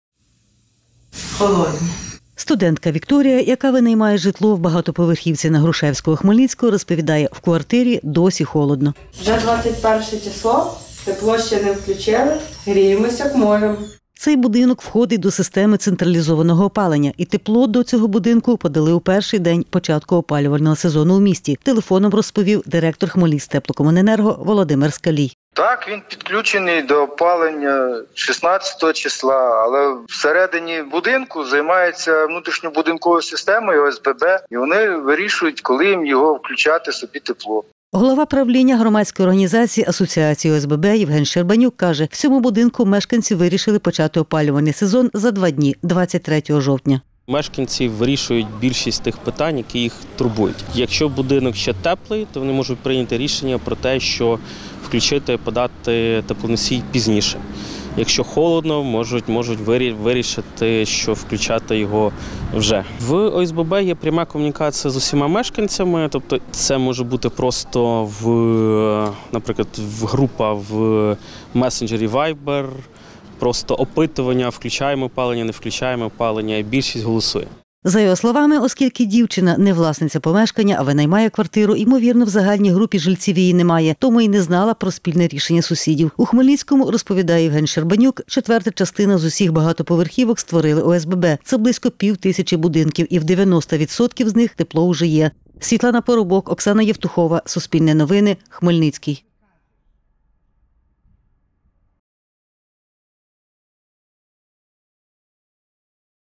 У Хмельницькому не у всіх мешканців квартир тепло
У більшості хмельницьких багатоповерхівок, мешканці яких створили ОСББ, тепло уже є, розповів голова правління «Асоціації ОСББ» Євген Щербанюк. Проте не у всіх. Від чого це залежить,  з’ясовували кореспонденти Суспільного.